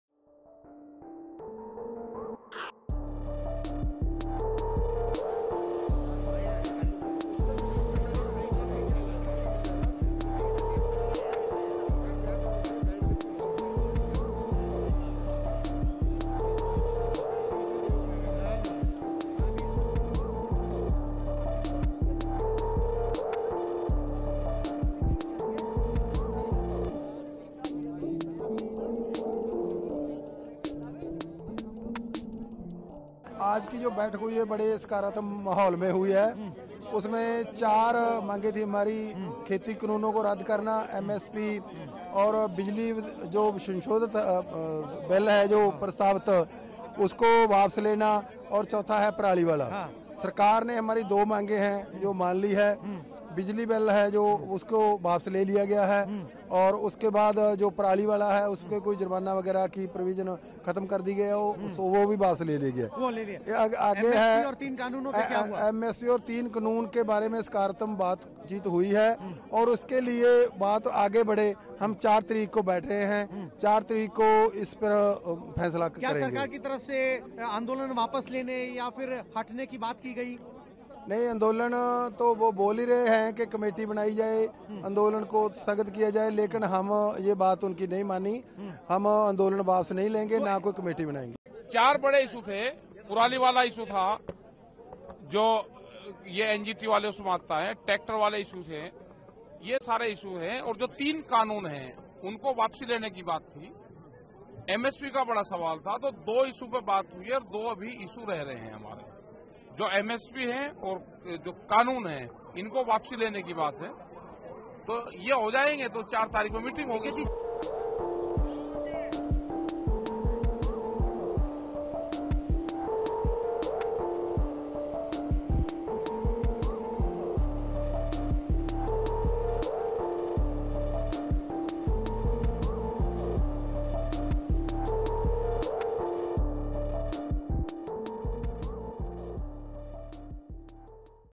Farmer leaders await next meeting with Centre after consensus on 2 issues
New Delhi, Dec 30 (ANI): After 6th round of talks with Central government at VigyanBhawan, farmers’ unions informed that they will discuss MSP and repealing of 3 farm laws in the next meeting on 4th of January. While speaking to ANI, Rakesh Tikait, Spokesperson, BharatiyaKisan Union (BKU) said, “Issues related to stubble burning and electricity were sorted in today's meeting. 2 of our main issues still need to be resolved. We'll discuss topics related to MSP and repealing of 3 Farm laws in the next meeting on 4th Jan.” Balkaran Singh Brar, Punjab President, All India Kisan Sabha said, “Talks were positive today. Govt has been saying that we should end agitation and form a committee. But we didn't listen to them. We won't take back our movement. We won't form any committee. We'll discuss MSP in next meet.” Earlier, Union Agriculture Minister Singh Tomar informed that consensus on 2 out of 4 issues was reached between both sides.